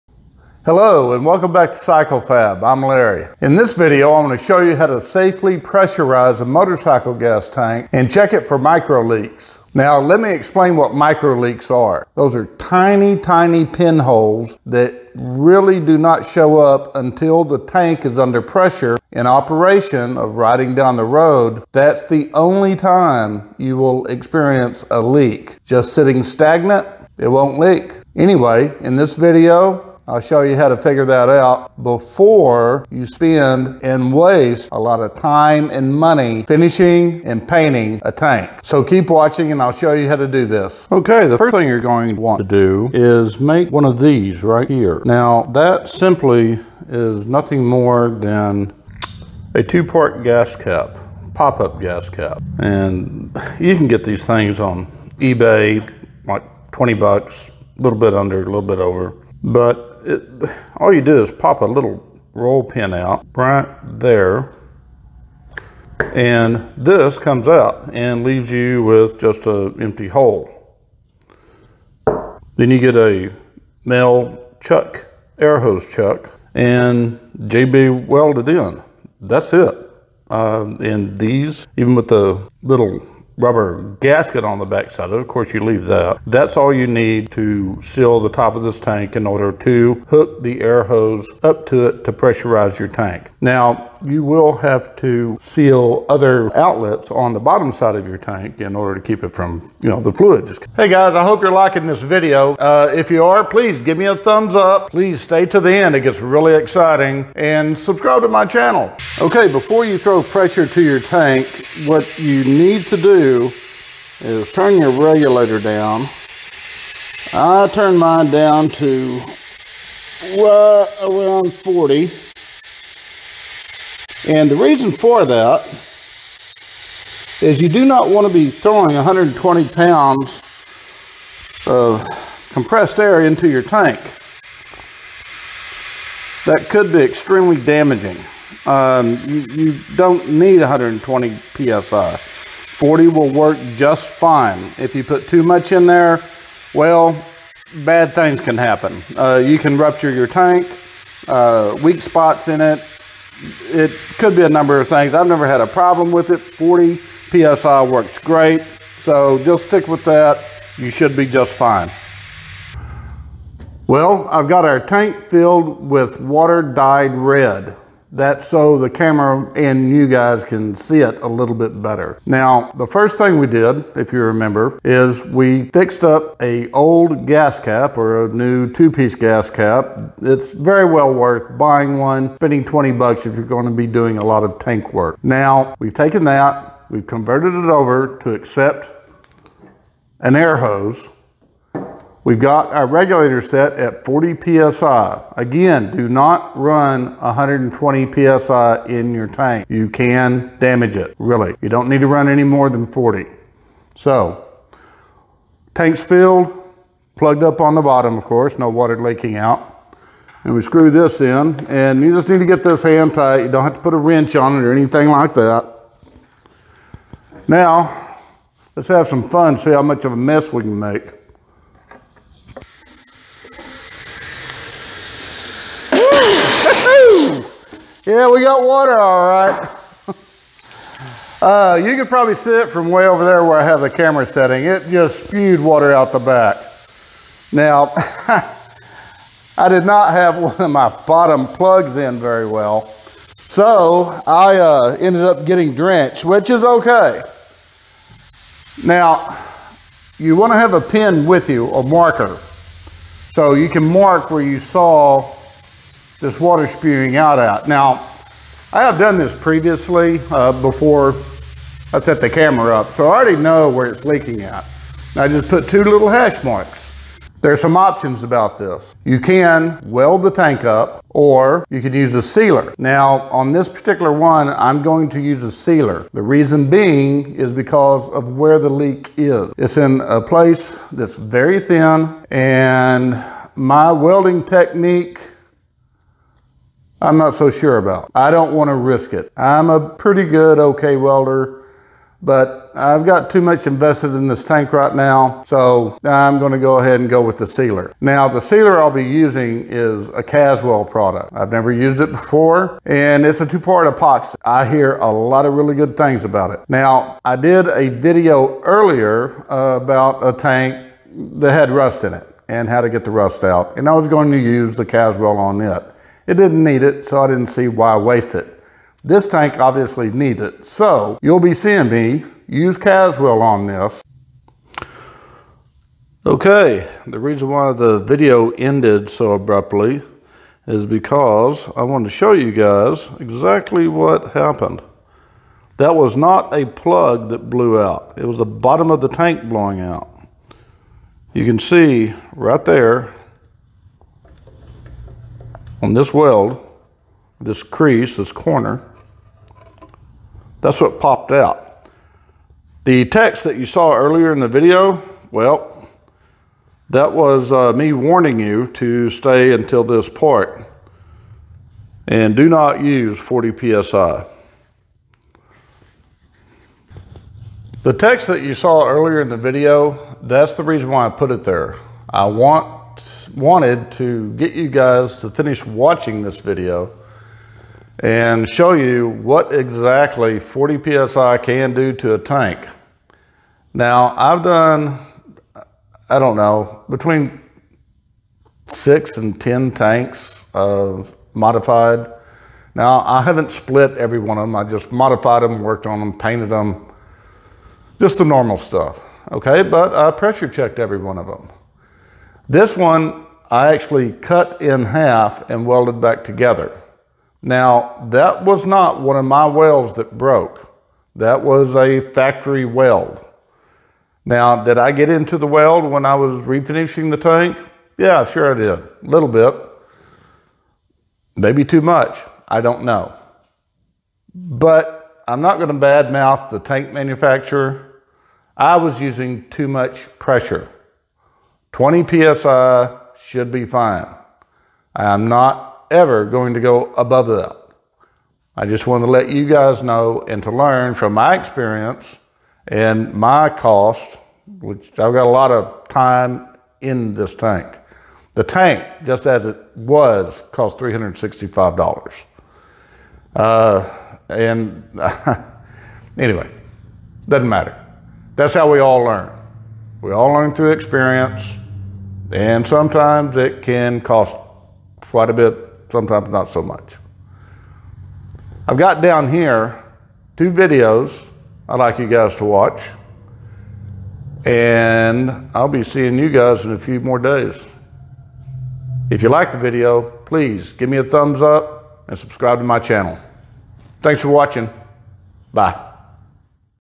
HOW TO (NOT) PREASURE CHECK A GAS TANK !
Here is a video of how not to pressure check a gas tank. Tanks can explode with not very much  P.S.I.